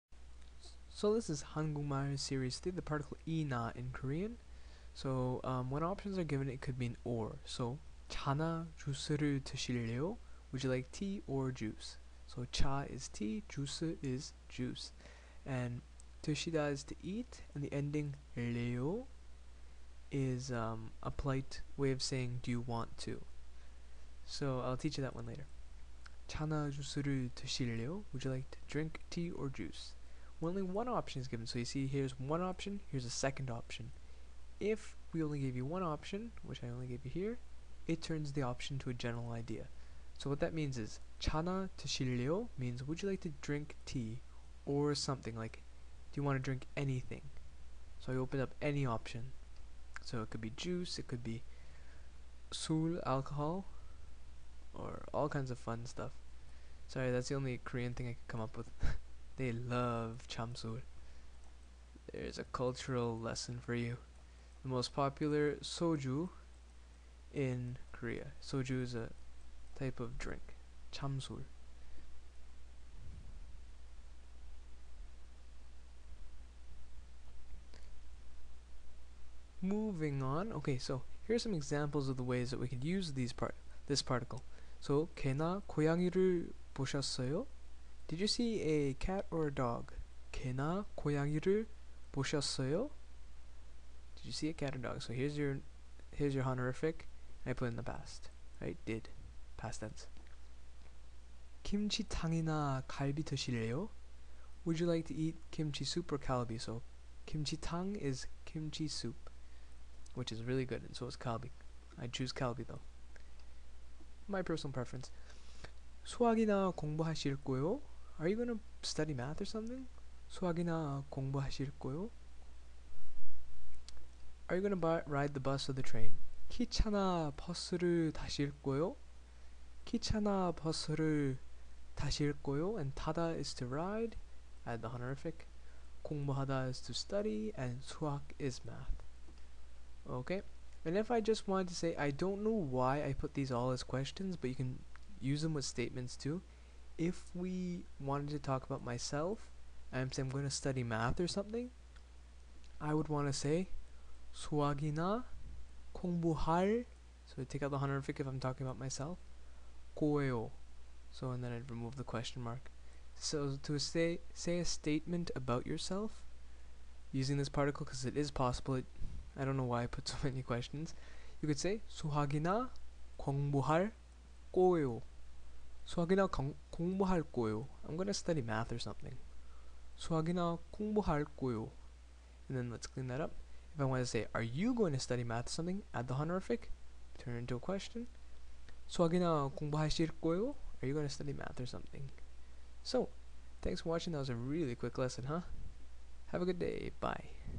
Lesson 33 - The Particle -(이)나
The particle 나 (이 gets attached if the last letter of the word is a consonant) attaches to words and means or.

차나 쥬스를 드실래요? Would you like tea (차) or juice(쥬스)?

Notice the normal particle can attach to the second word.

If there's only one word with (이)나 attached, it generalizes the word, usually part of a suggestion.

차나 드실래요? Would you like tea (OR SOMETHING ELSE TO DRINK)?